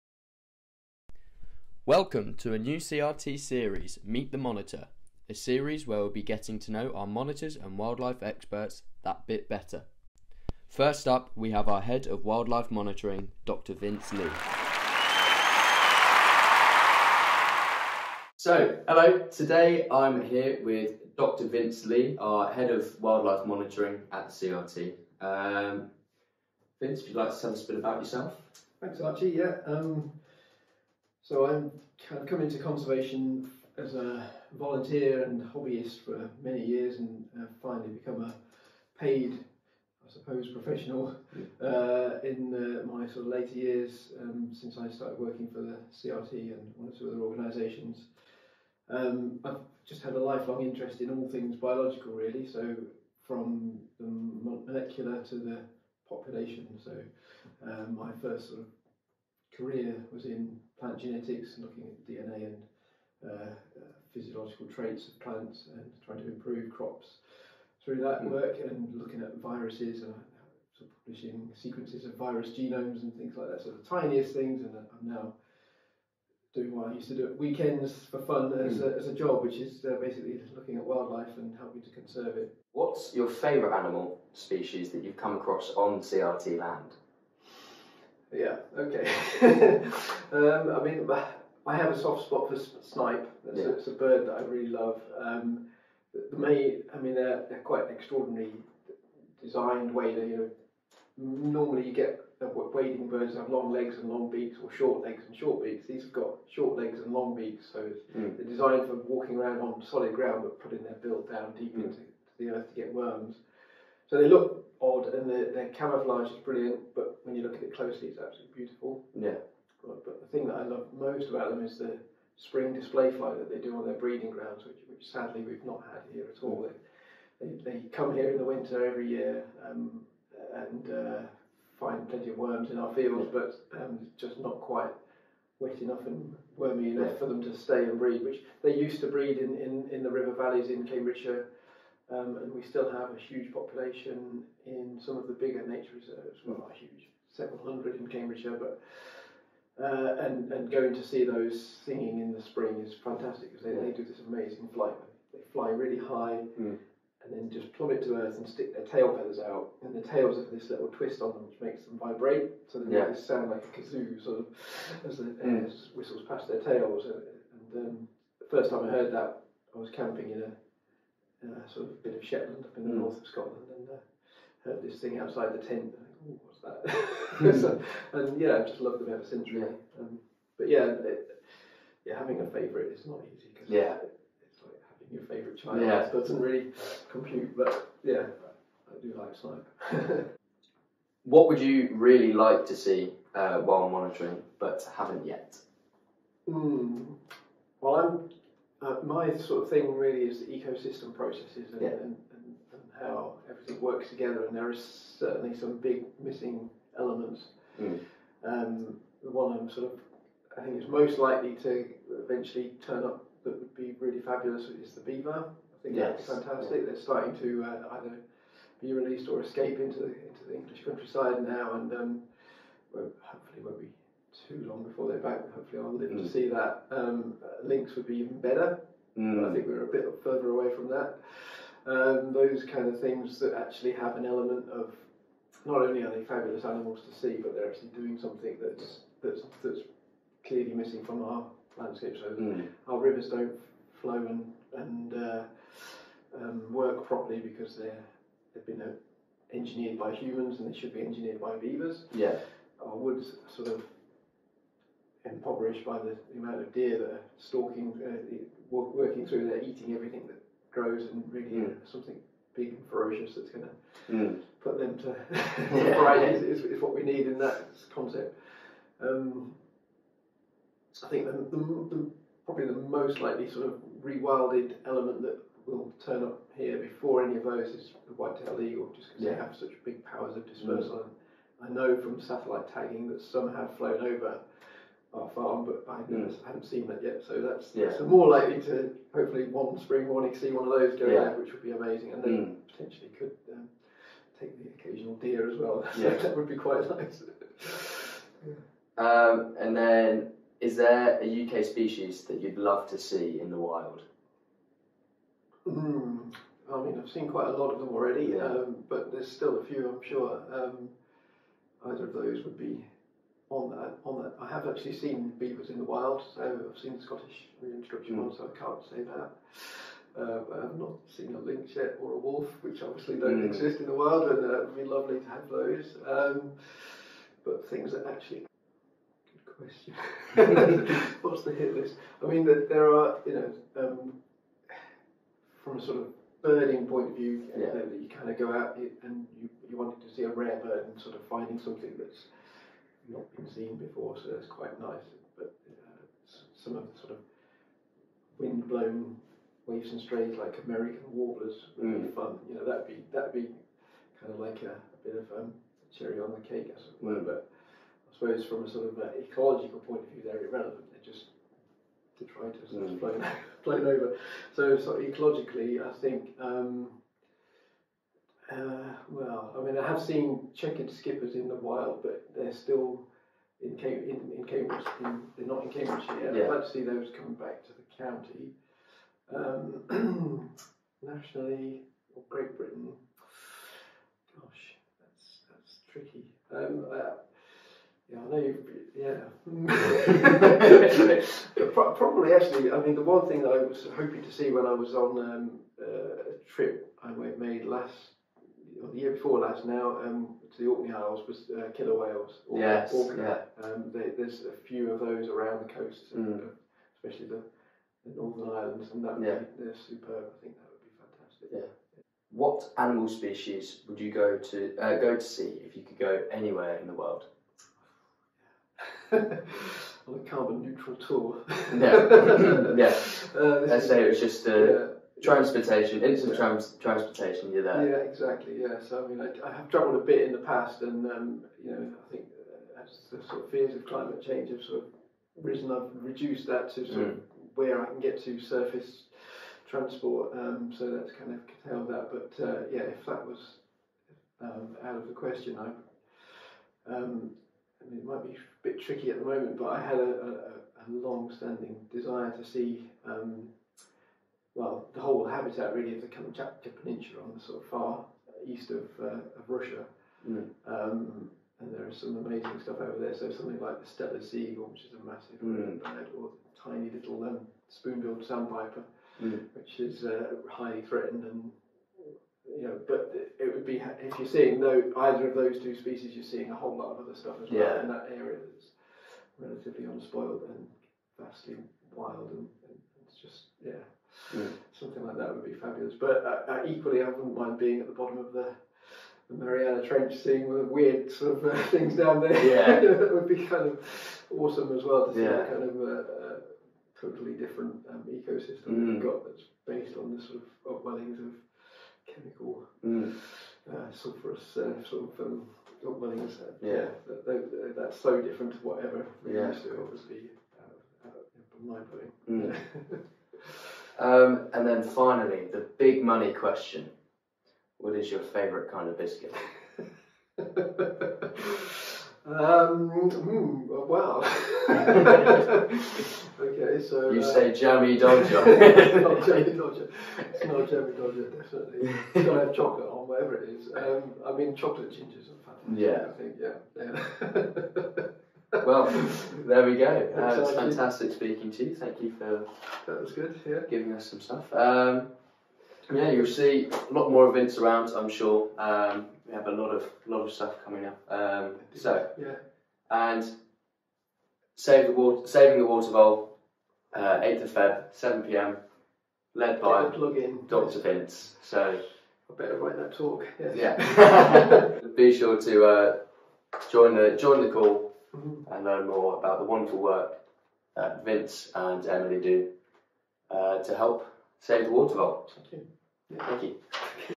Meet the Monitor - Dr Vince Lea
Meet Dr Vince Lea, our Head of Wildlife Monitoring. Find out about his wildlife journey with the CRT and what’s on his monitoring wishlist.

Join Vince on Wed 8 Feb for a FREE online talk on Saving the Water Vole at 7pm